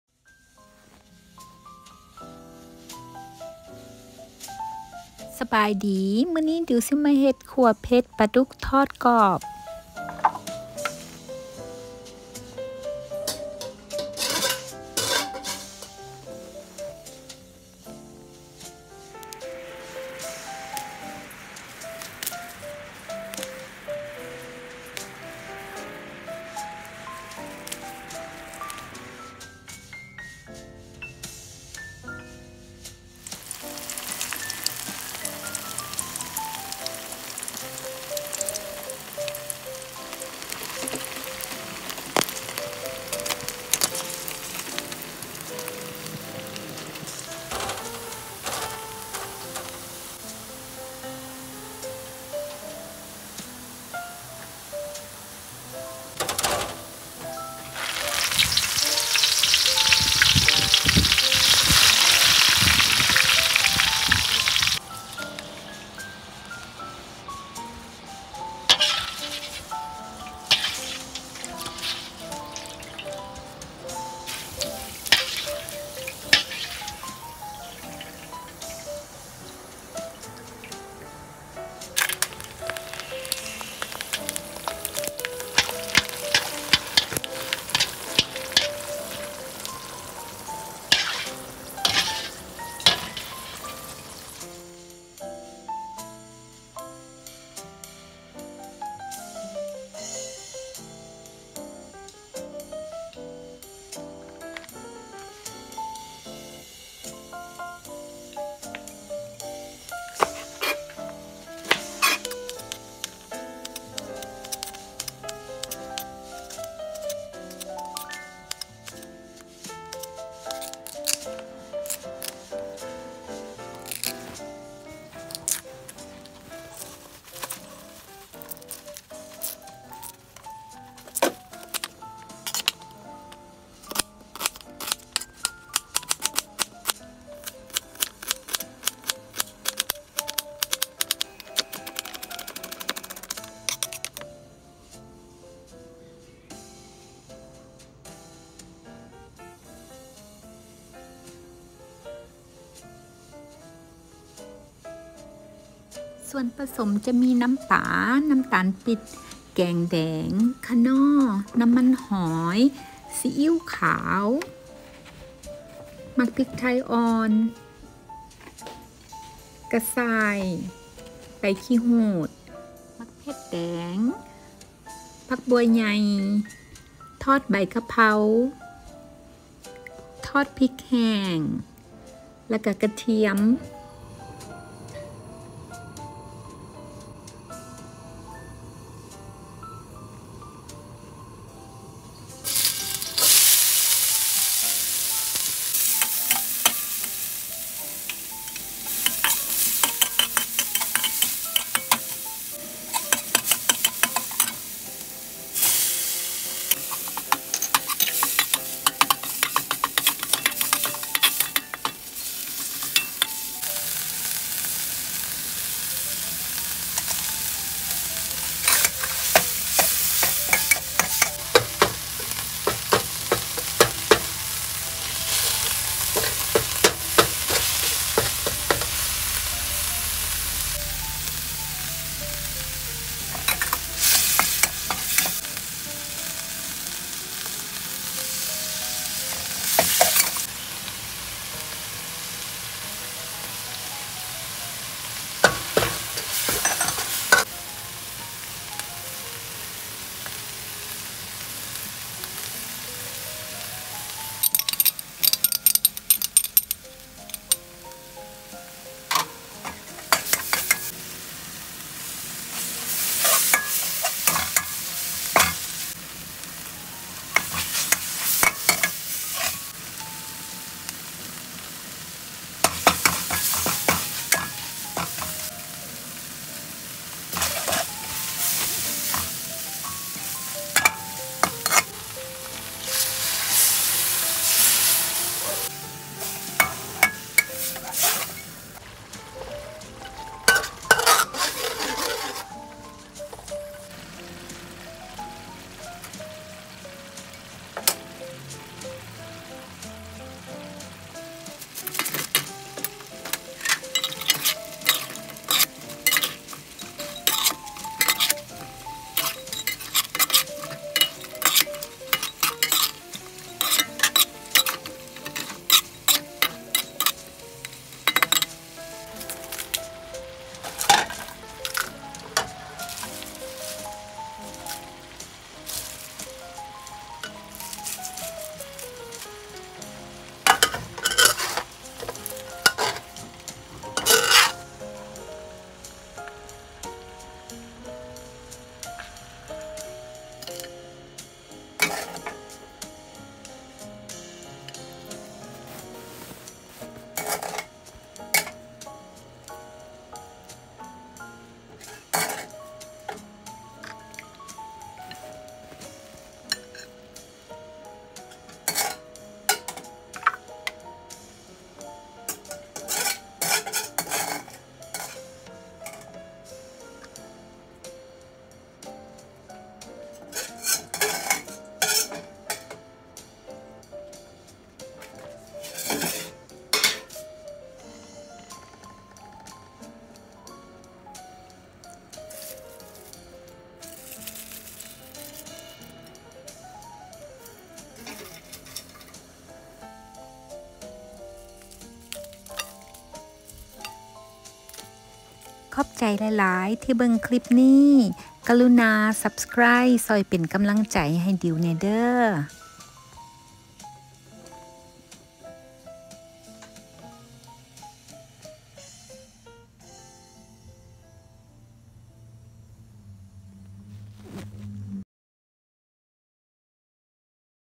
How to Cook Fried Spicy Crispy Catfish วิธีทำผัดเผ็ดปลาดุกทอดกรอบ ວິທີເຮັດຂົ້ວເຜັດປາດຸທອດກອບ
Ingredients:

Young green pepper
Holy basil 🌿
Dried chili
Fingerroot. Aka.Krachai
Kaffir lime leaves
5 cloves of garlic 🧄 minced
1/4 chopped of onion 🧅
Slice fresh chili 🌶
1 can red curry paste
1 tsp of White soy sauce
2 tbsp of Oysters sauce
1 tsp of Fish sauce
2 tbsp Palm Sugar /Brown sugar
1 tbsp of Olive oil
1 tbsp of chicken soup base mix
1.5 cups of water

วัตถุดิบ:

 กระชายซอย
 พริกไทยสด
 ใบกะเพรา 🌿
 พริกแห้ง
 กระเทียม 5 กลีบ 🧄 สับละเอียด
 หอมใหญ่หั่น 1/4 🧅
 พริกสดฝาน 🌶
 น้ำพริกแกงเผ็ด 1 กระป๋อง
 น้ำตาลปี๊บ 2 ช้อนโต๊ะ / น้ำตาลทรายแดง
 น้ำมันมะกอก 1 ช้อนโต๊ะ
 ผงปรุงรส 1 ช้อนโต๊ะ
 น้ำเปล่า 1.5 ถ้วย